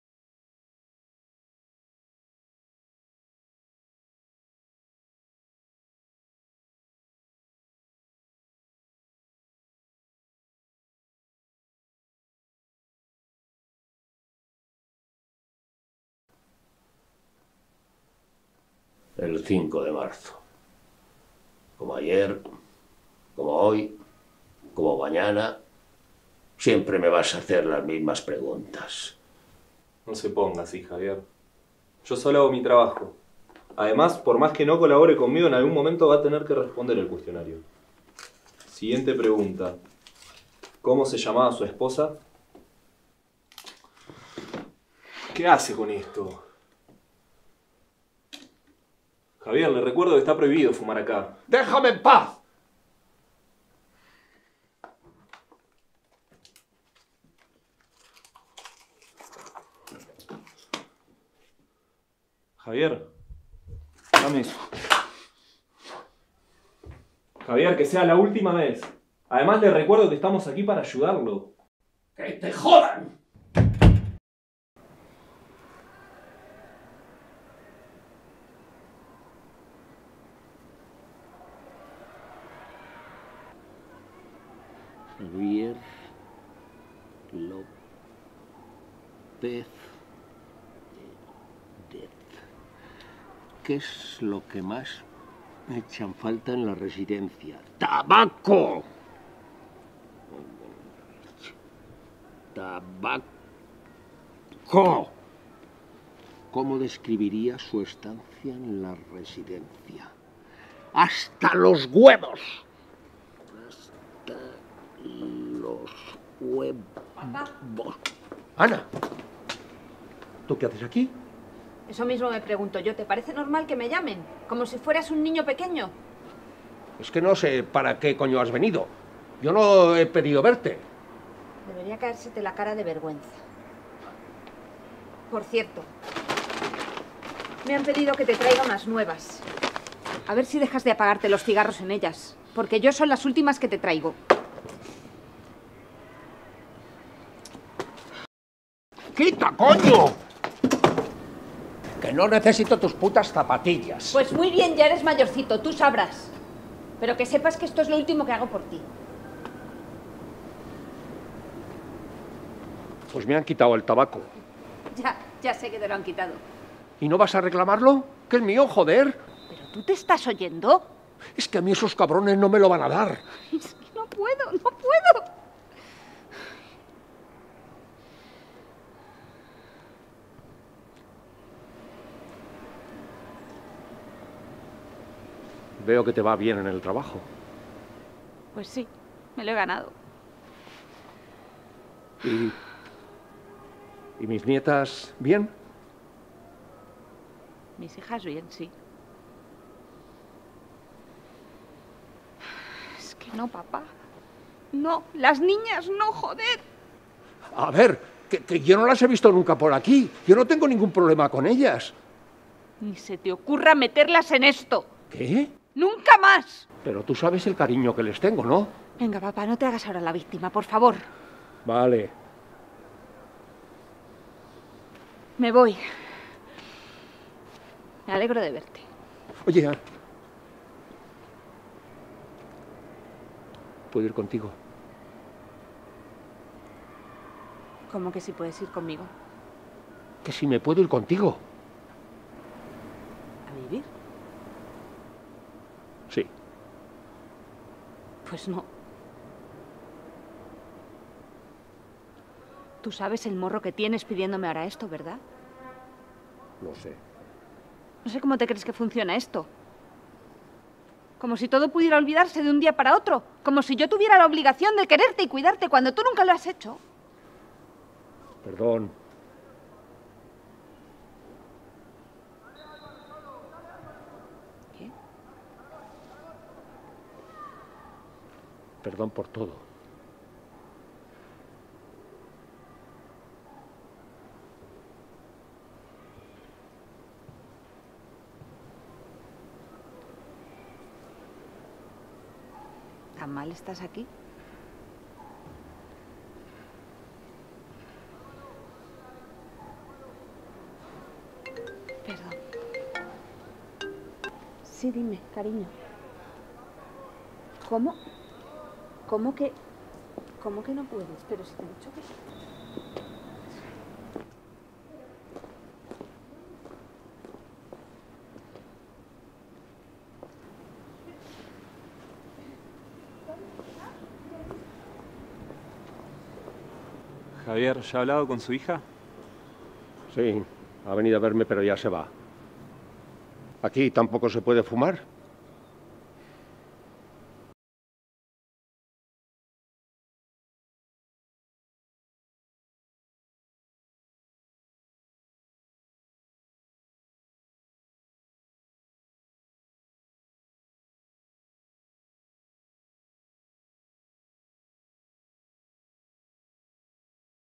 CORTOMETRAJE - "Javier" [ESP]
Este es mi primer cortometraje significativo. También fue para una asignatura de la universidad, impartida en ese momento por el ganador del Goya David Pérez Sañudo.

La historia gira en torno a Javier, un hombre mayor gruñón y orgulloso que no se adapta del todo a la residencia de mayores que ahora es su hogar. En un último intento desesperado, le pedirá a Ana, su hija, poder irse a vivir con ella, a sabiendas de que a vida de Ana es muy complicada con un trabajo mal pagado, un divorcio en trámite y dos niñas. Esta es la realidad de muchas personas, y la visión de rechazo de la sociedad ante las residencias de mayores se ve desafiada por la decisión final de Javier entre lo que desea y el sacrificio que no complicará más la vida de su única hija.

Siendo el primer trabajo que tanto yo como mis compañeros hacíamos de este estilo con actores reales y localización específica, encontramos varios errores en diferentes áreas y etapas del mismo. Aún así, yo lo considero uno de los posibles muchos cortometrajes que quedan por hacer y experiencias por vivir.

Muchas gracias al elenco por ser tan profesional y divertido en todo momento:
Paco Angulo como JAVIER.
Belén Chanes como ANA.
Juliano Gallo como ENFERMERO.

Y también a todo el equipo técnico:
Mariangela Bañeras, Diego de La Rosa, Lucía García-Comas, Andrea Guerrero, Carlos Yubero, Martín Caballero, y mi persona Gabriela Barón Medina.